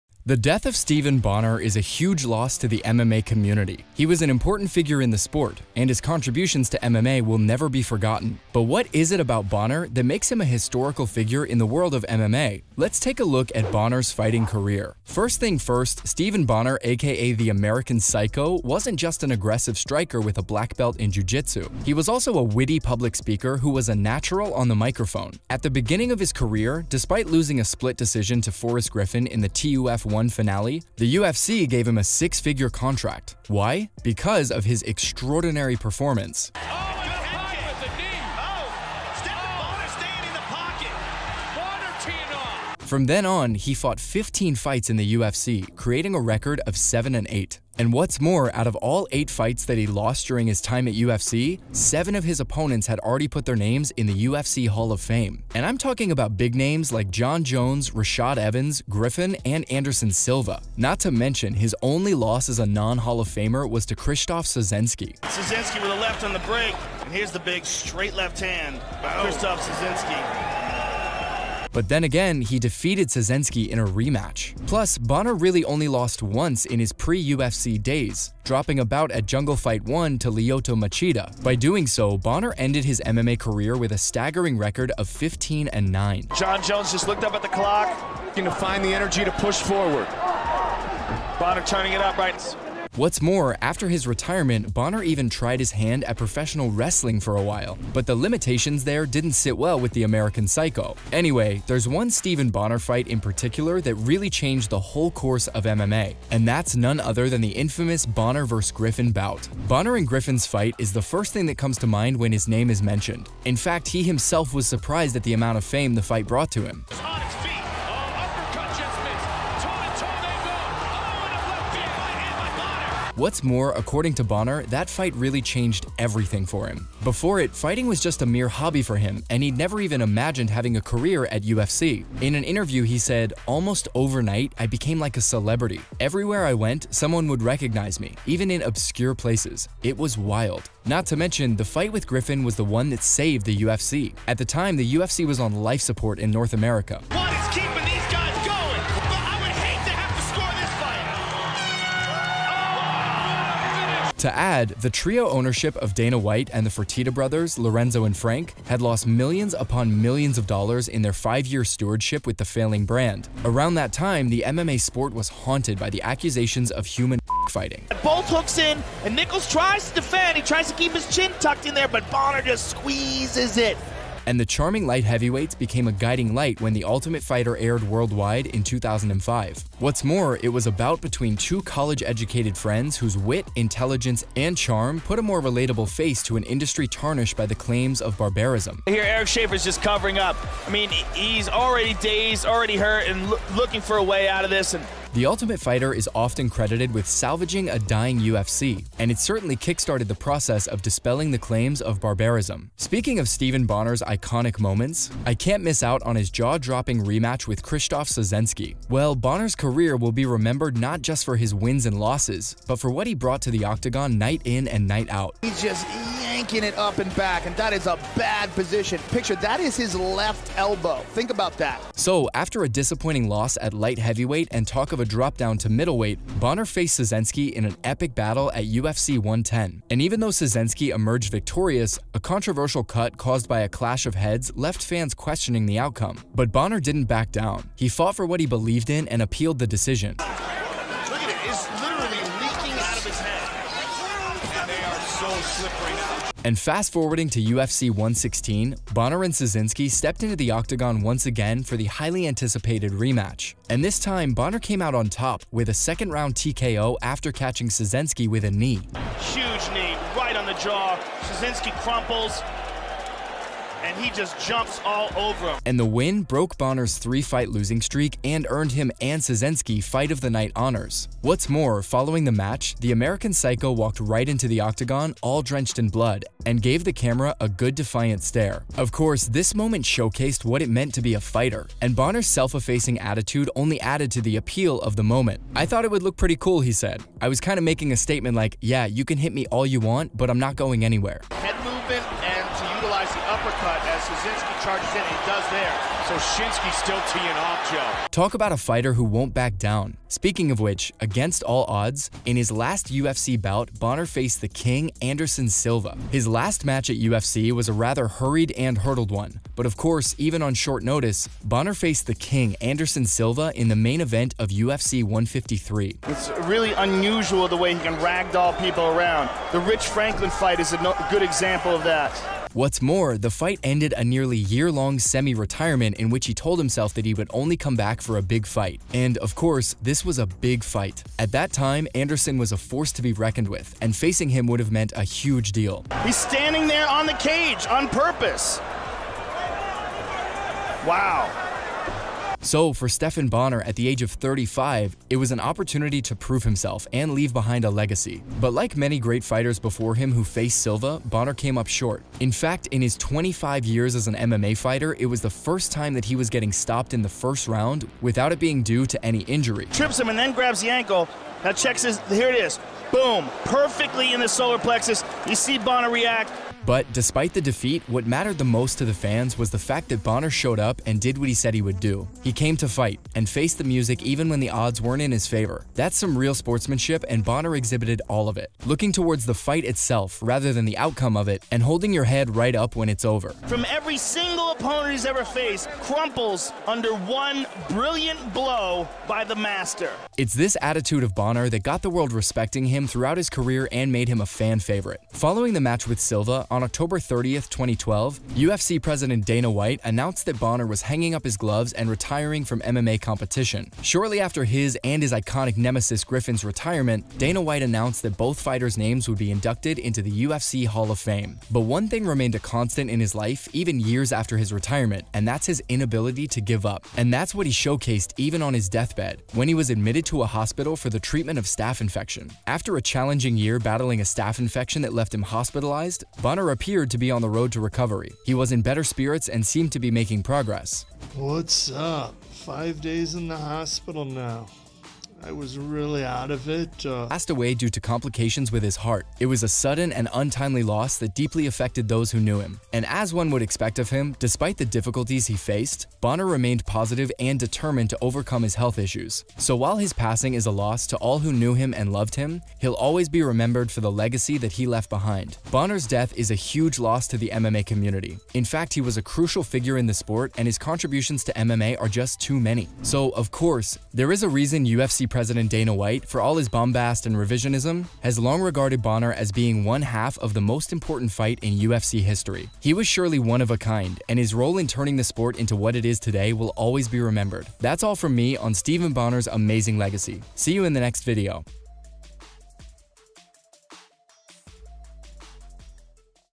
Why Stephan Bonnar IMPORTANCE To MMA Should NEVER Be Forgotten..
Welcome back to Encore Sports, today on the channel we're going to be talking about "Why Stephan Bonnar IMPORTANCE To MMA Should NEVER Be Forgotten.." if you're excited to learn more about this then make sure you watch the whole way through because you don't want to miss the details we have for you!